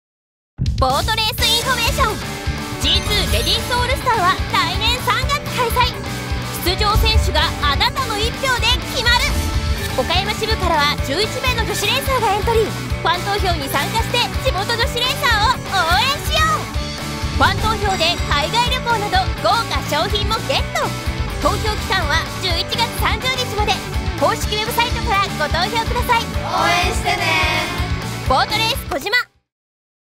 ＧⅡ第３回 レディースオールスター ファン投票 TVCM30秒
ボートレース児島で開催される「ＧⅡ第３回 レディースオールスター」のファン投票TVCMです。

ボートレース児島 公式Facebook
アカウント▶▶@boatrace.kojima.jp